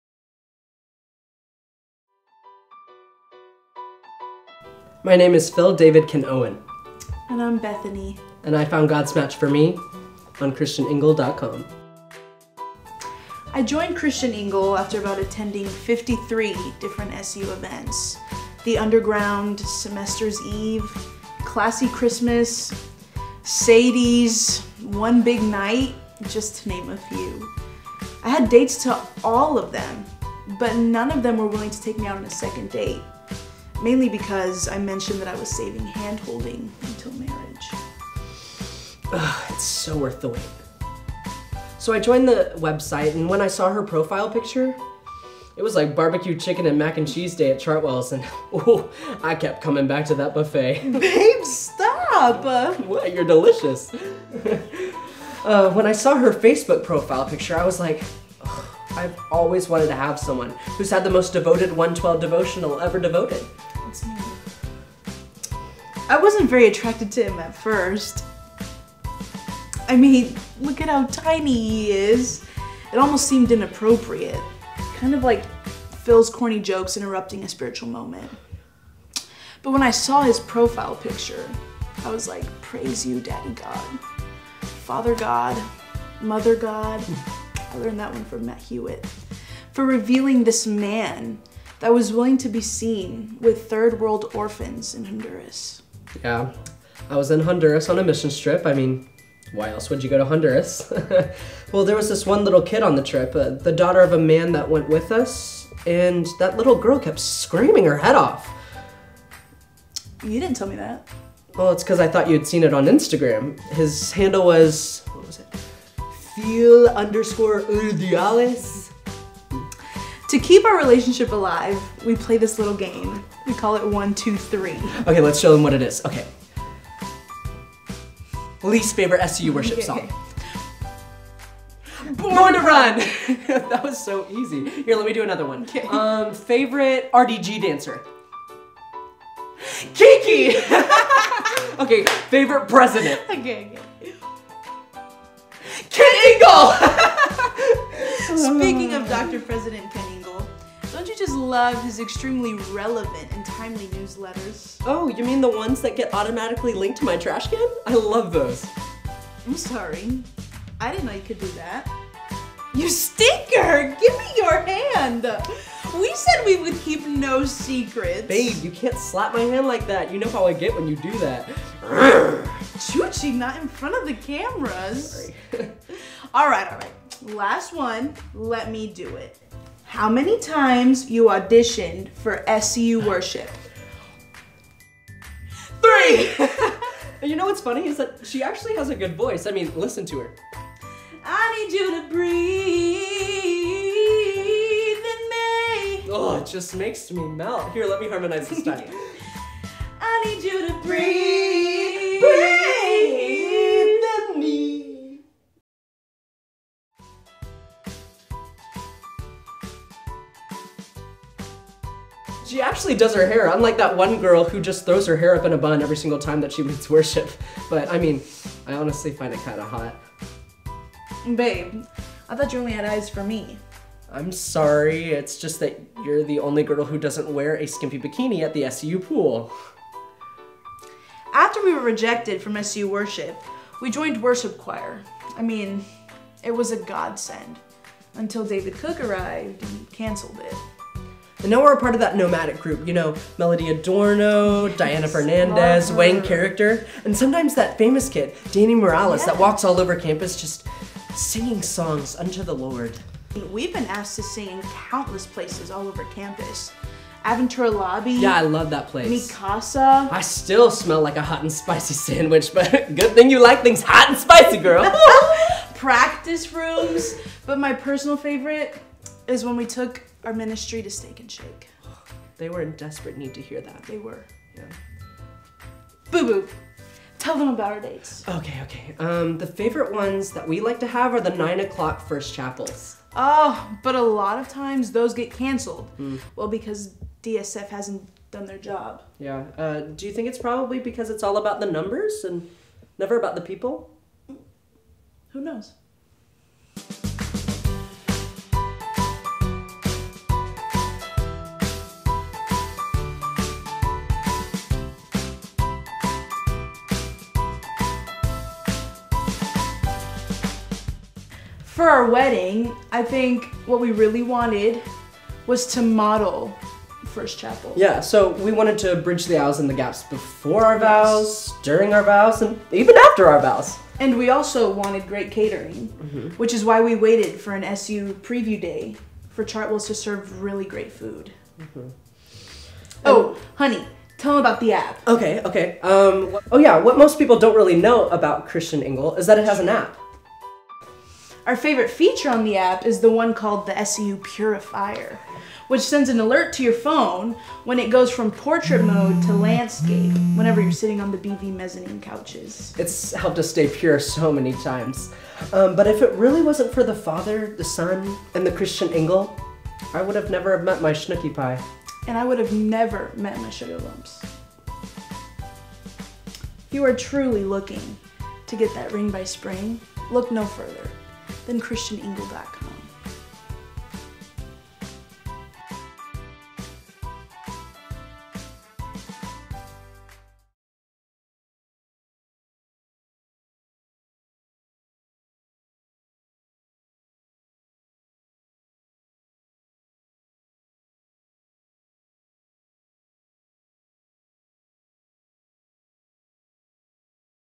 ChristianIngle
An SEU TNL Video (Kara Hawayek & Benjamin Peacock)

Written by: Ashley Espada, Kara Hawayek, & Benjamin Peacock
Filmed by: Evin Howell, Patrick Munoz, & Taylor...

All ideas were inspired by ChristianTingle (Tripp & Tyler) and SEU's shortcomings.